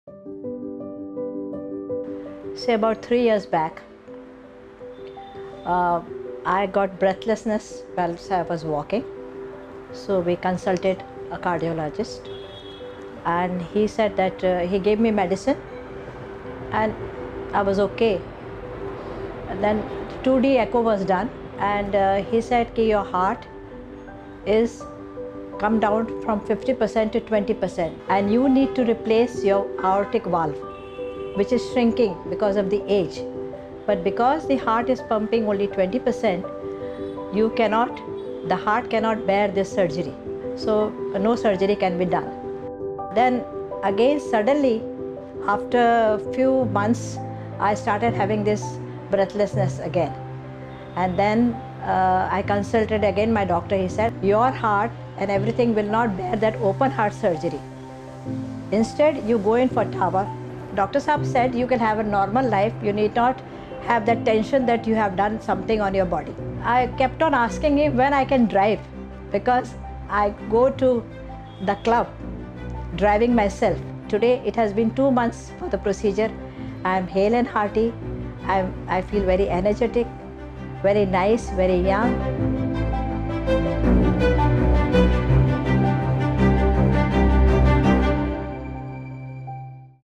Recovering from Aortic Stenosis with Transcatheter Aortic Valve Replacement (TAVR) | Medtronic India
About 3 years ago I used to suffer from constant breathlessness. After a 2D echocardiography test, I got to know that my aortic valve was shrinking and had to be replaced. I was then suggested to undergo the Transcatheter Aortic Valve Replacement therapy as my heart was too weak for other major surgeries. Post the treatment, I started walking in just a couple of days and was also able to resume my regular activities in a few months.
- Mrs. Aruna Shah (Aortic Stenosis Survivor)

About the treatment:
Aortic stenosis is a chronic heart condition which occurs due to the failing of the aortic valve. This chronic condition can be treated through a minimally invasive procedure called Transcatheter Aortic Valve Replacement therapy (TAVR). The procedure is done using an artificial heart valve. The device is a metal frame made with a blend of nickel and titanium which is placed inside the heart to replace the diseased, malfunctioning valve. This metal frame device shapes itself according to one’s heart structure and sets the blood flow back to normal.